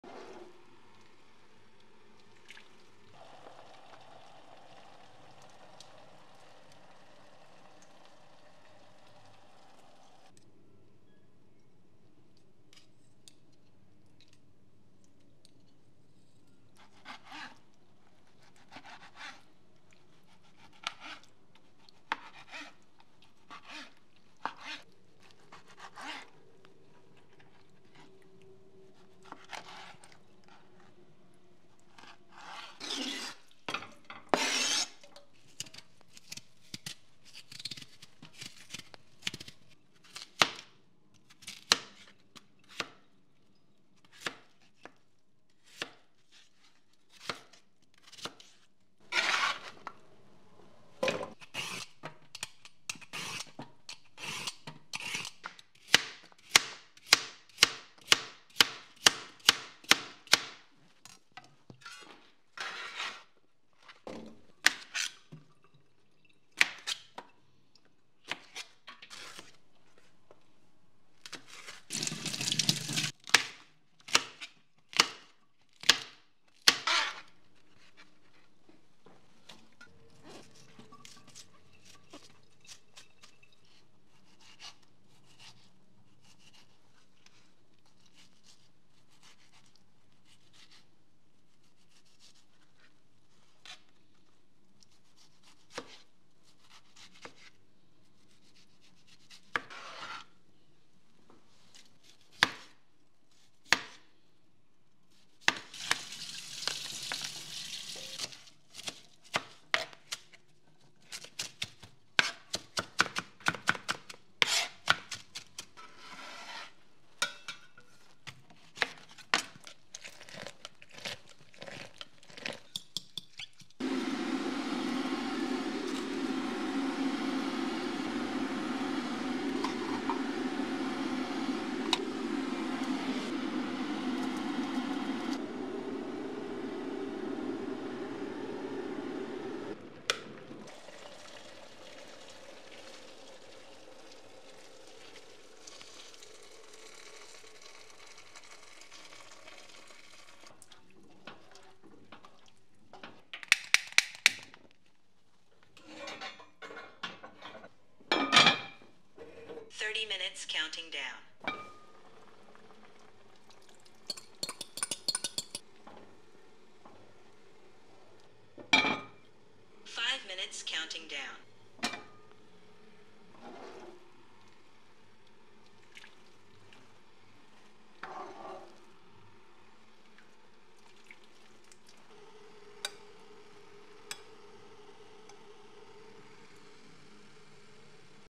Salmon Soup | Shelter's Cuisine
Fish soup with salmon, potato, carrot, tomato and green pepper.